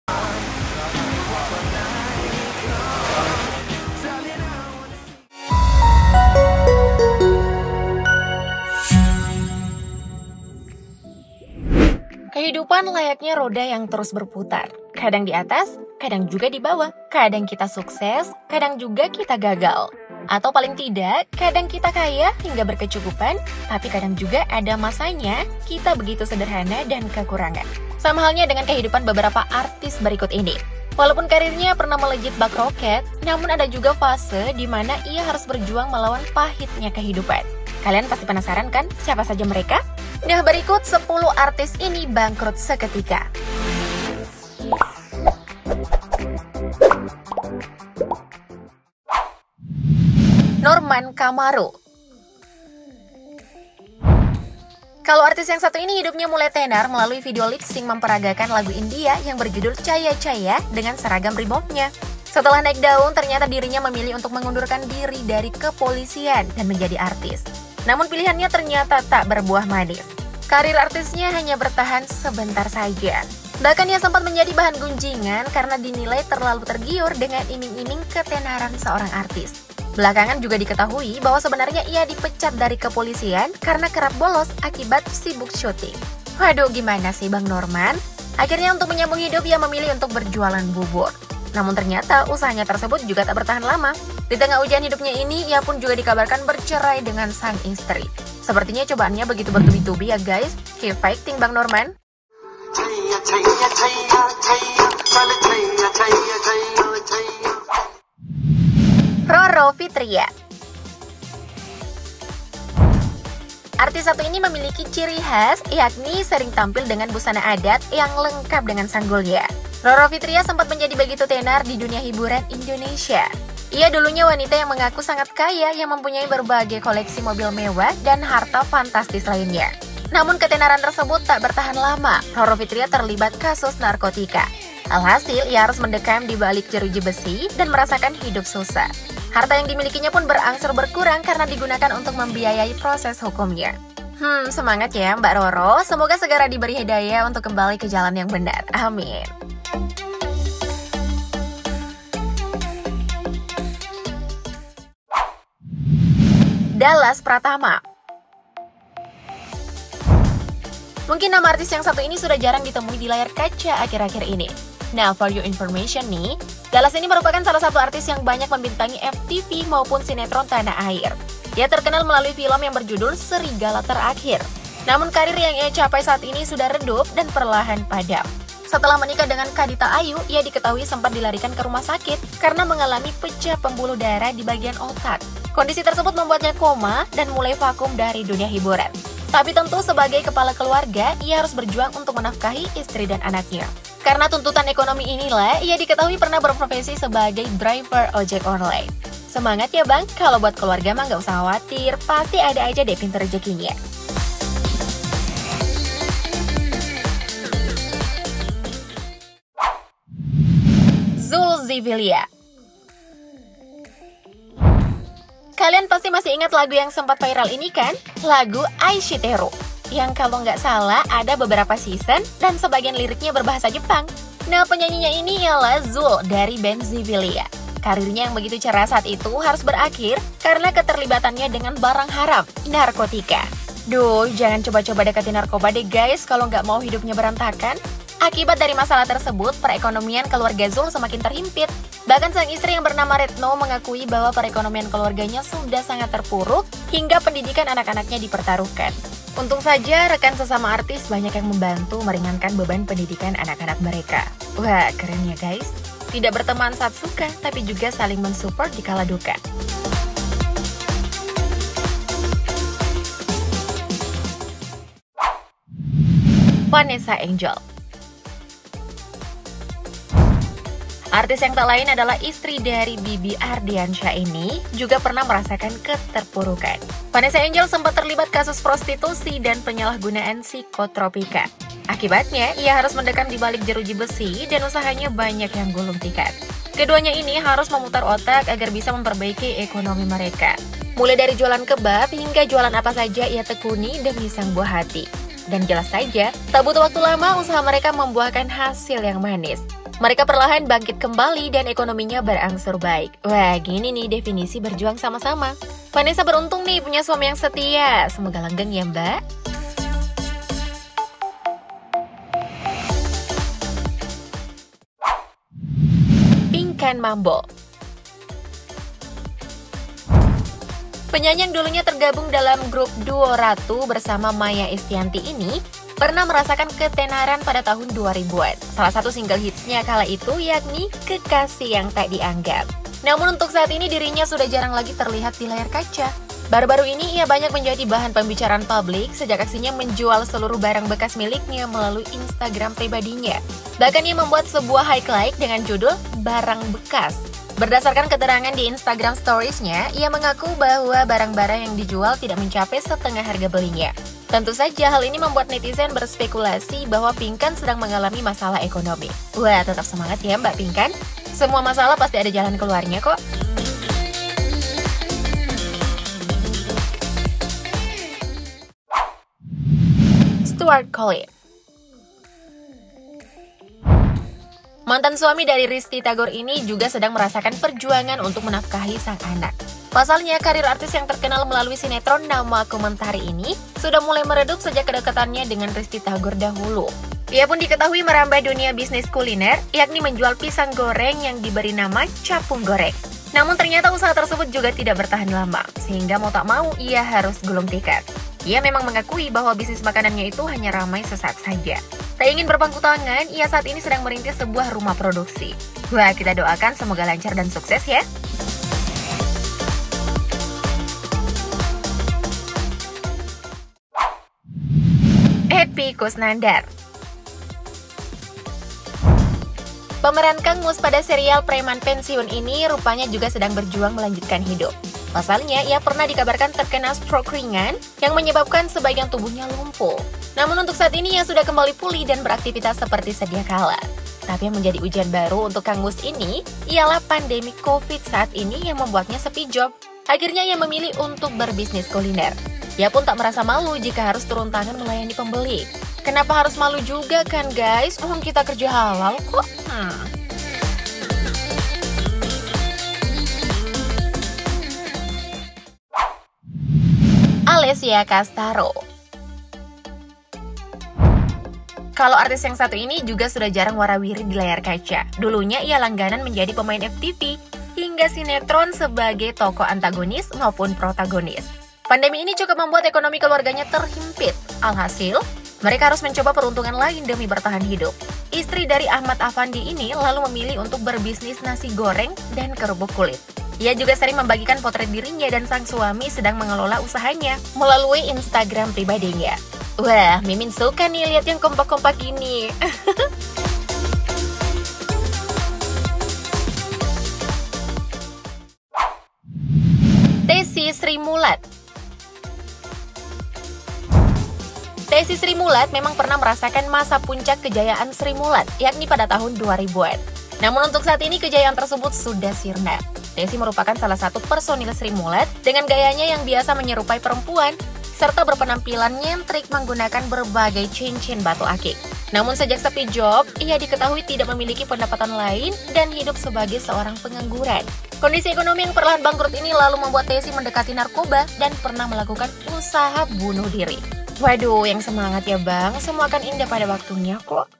NASIBNYA SUNGGUH MIRIS! 10 Artis Ini Bangkrut Seketika Karena Terlalu Bebas dan Salah Pergaulan
Kehidupan layaknya roda yang terus berputar. Kadang di atas, kadang juga di bawah. Kadang kita sukses, kadang juga kita gagal. Atau paling tidak... Kadang kita kaya hingga berkecukupan, tapi kadang juga ada masanya kita begitu sederhana dan kekurangan. Sama halnya dengan kehidupan beberapa artis berikut ini. Walaupun karirnya pernah melejit bak roket, namun ada juga fase dimana ia harus berjuang melawan pahitnya kehidupan. Kalian pasti penasaran kan siapa saja mereka. Nah, berikut... 10 Artis ini Bangkrut Seketika.